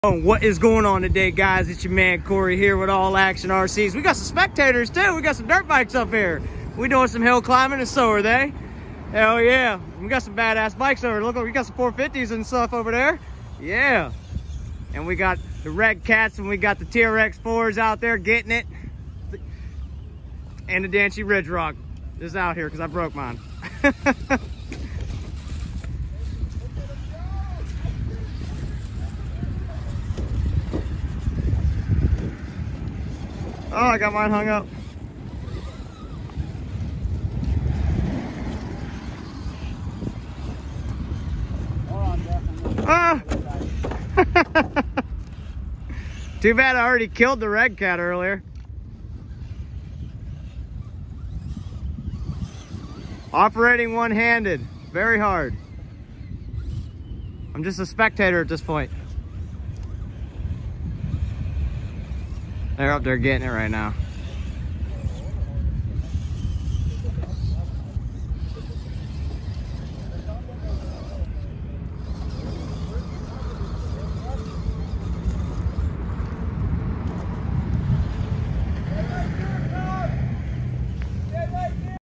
Rc Rock Crawling At Yellow Creek Wellsville
Rc Rock Crawler trucks introduced my danchee ridgrock we call the danche bus which will be painted soon